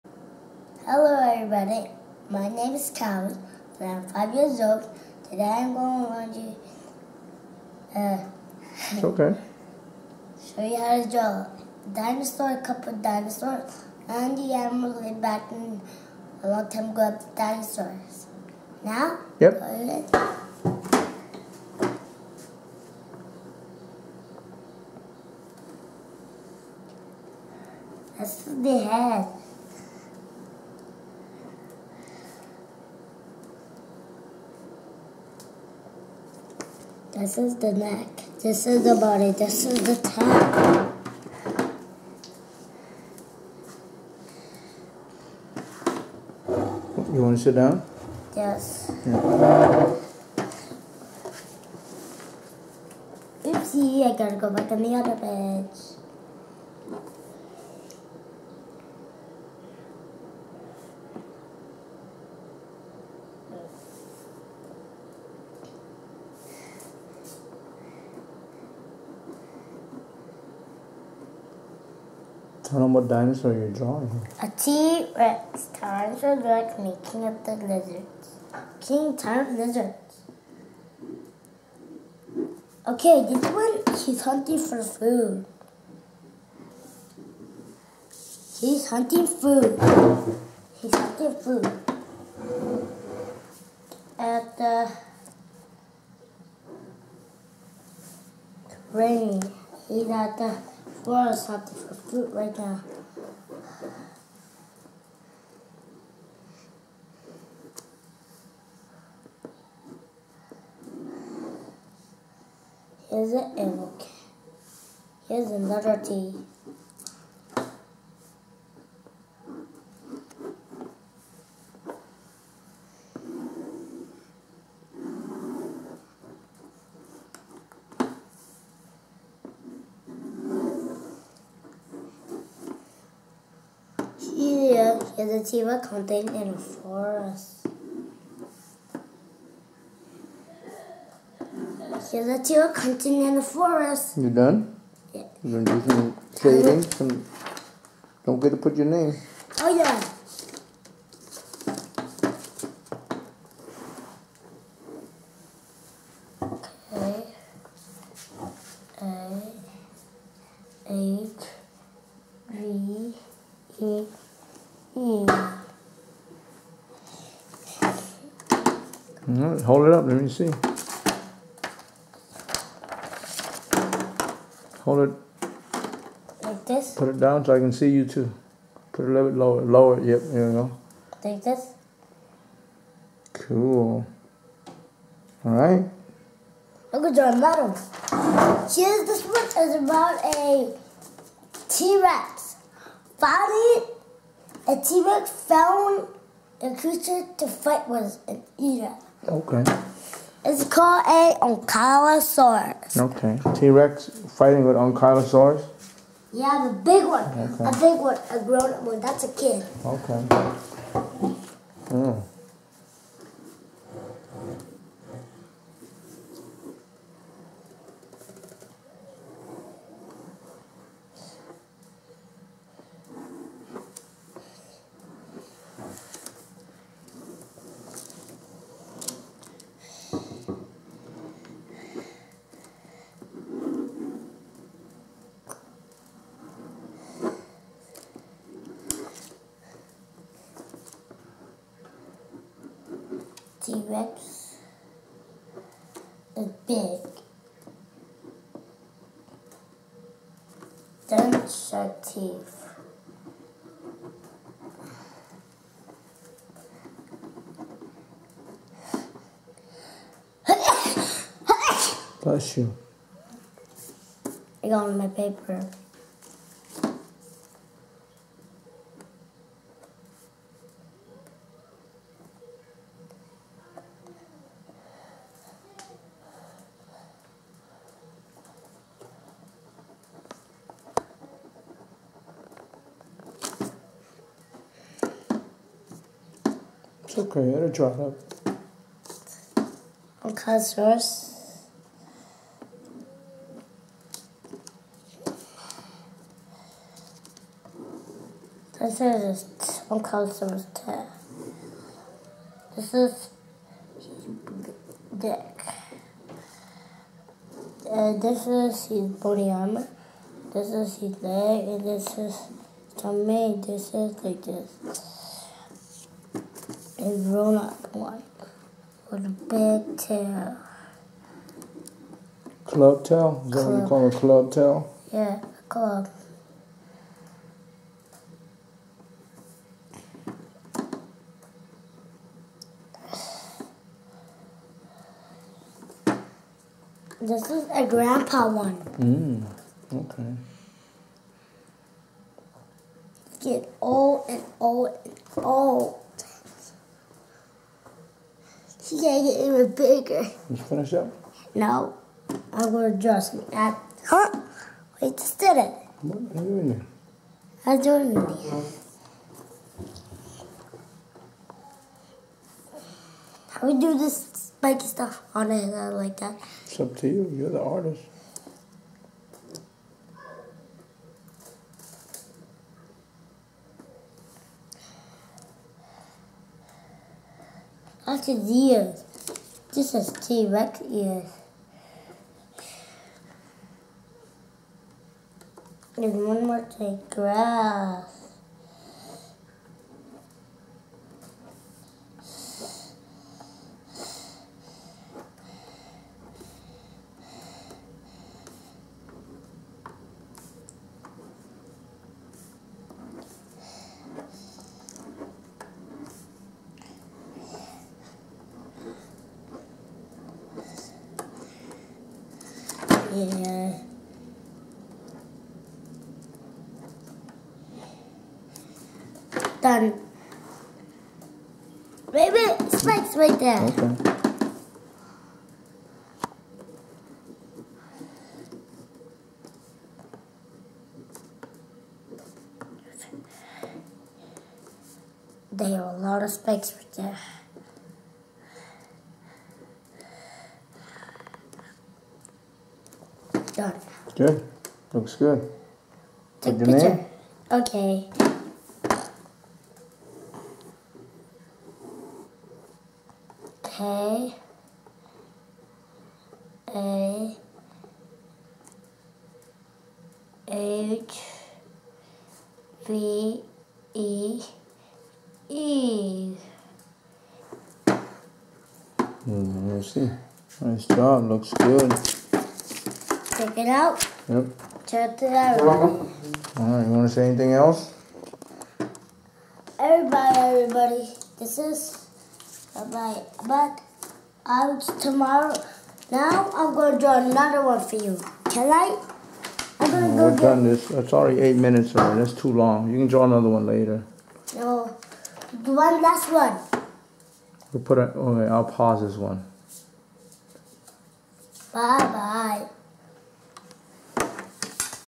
Draw T-Rex Hunting and T-Rex vs Ankylosaurus Drawn by Kahvee Lin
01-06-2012  How to draw T-Rex Hunting and T-Rex vs Ankylosaurus drawn by a 5 year old.